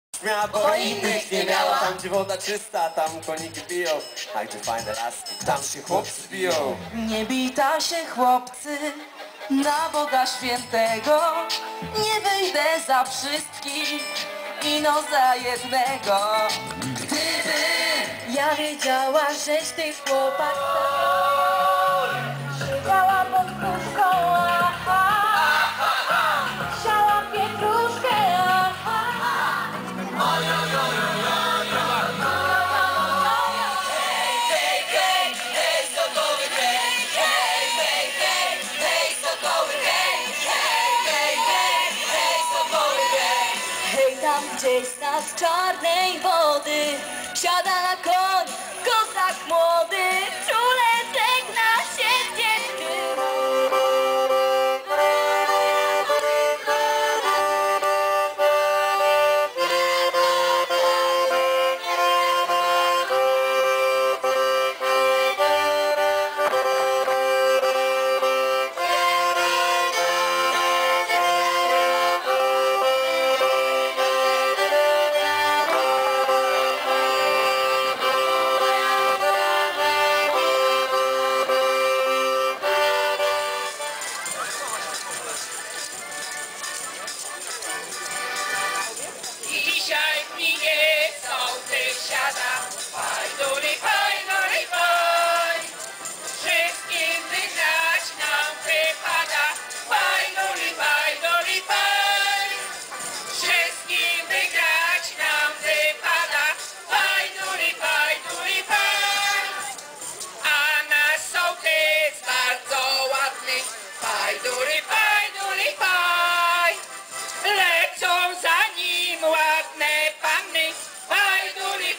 Dożynki w Budrach (pow. Węgorzewski) - 11 Września 2011
Koncerty, występy taneczne zespołów ludowych, a przede wszystkim, rywalizacja sołectw z powiatu węgorzewskiego czyli „Sołtysiada" były atrakcjami tegorocznych dożynek powiatowych w Budarch.
Słoneczna pogoda przyciągnęła tłumy gości na boisko gminne w Budrach, gdzie usytuowana była scena. Przybyli mogli zobaczyć i usłyszeć m.in.: „Sukces" z Mrągowa, Zespół Pieśni i Tańca Ełk, kapele folkową „Vieś Nova" z Suwałk, „Berkut" z Olsztyna, grupę „Absolwent", zespół „Alebabki" oraz Force Fire.